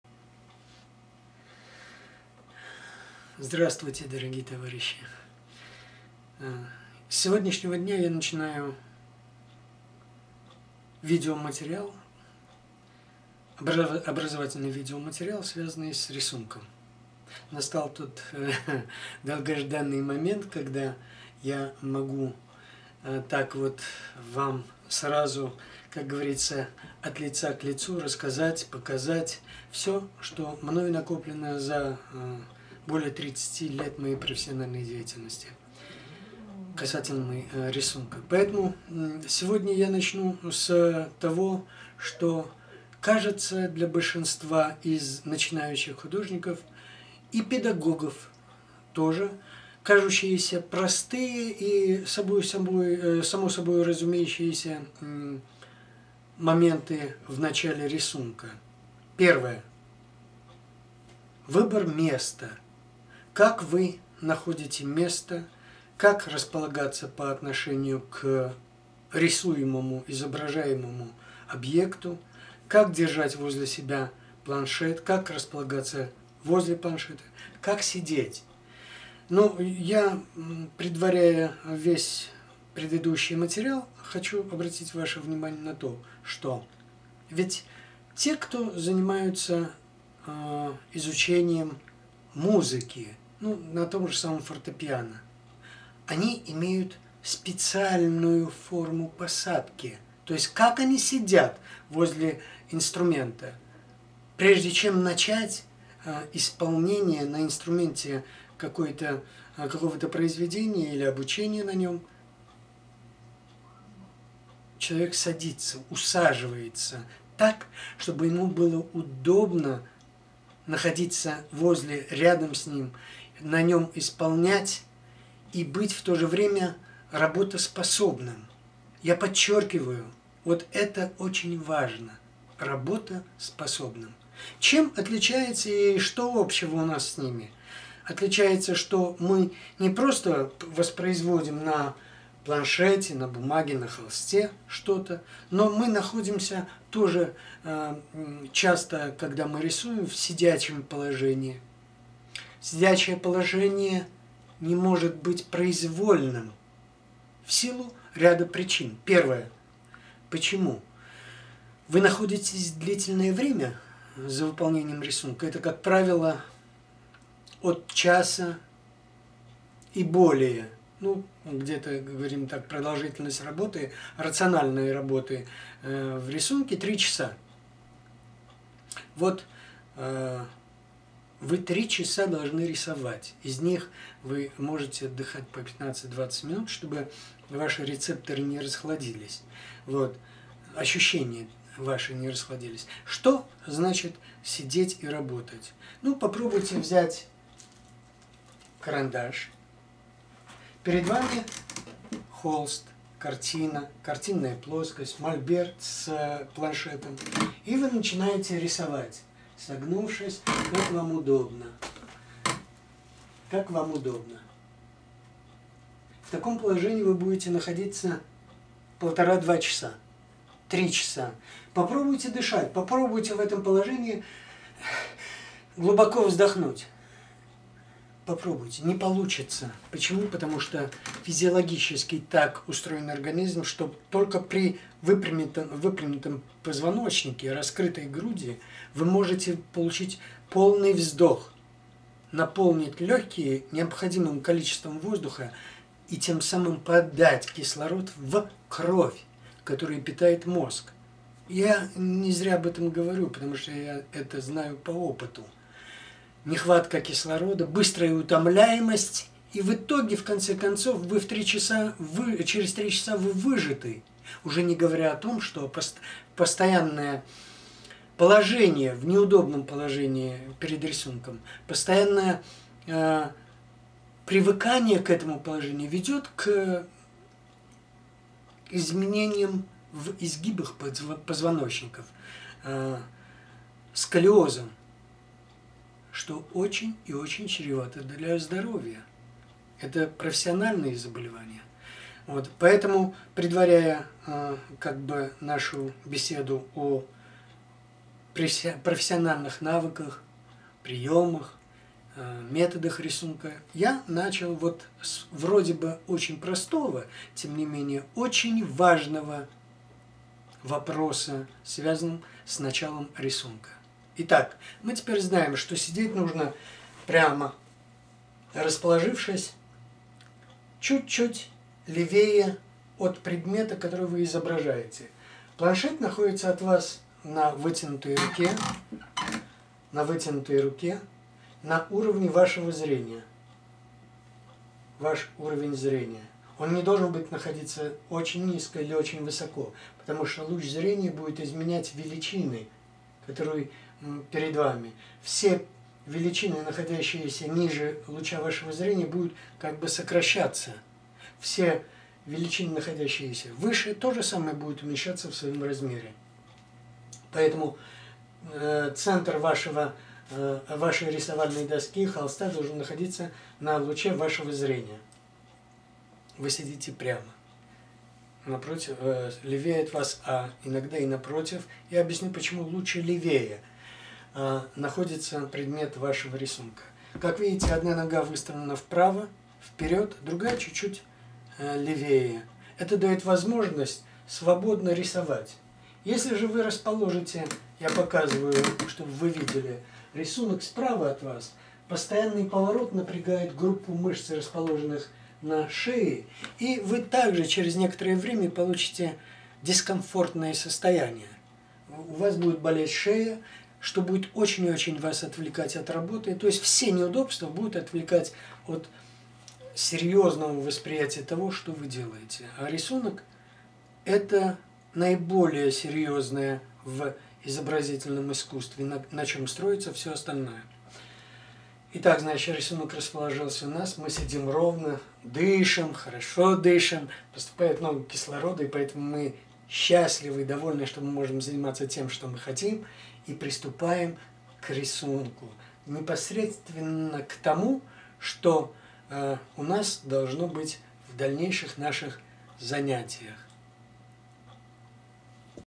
Before You Start Drawing
Are you ready to start drawing? Learn about the key points in the drawing process that are often missed by students and professionals alike.